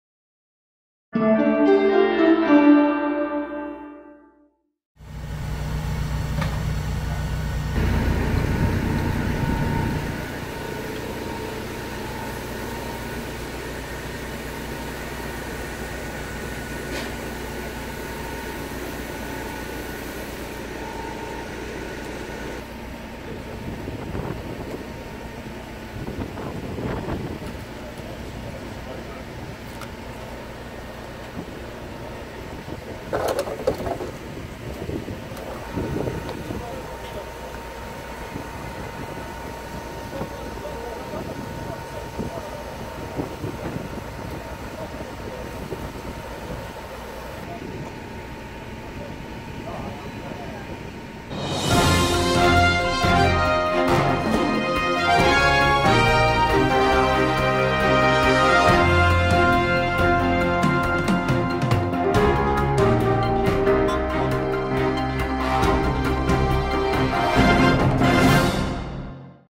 ctnews.ro | Incendiu auto în Constanța
Un autoturism parcat a luat foc, iar flăcările au afectat și clădirea lângă care se afla.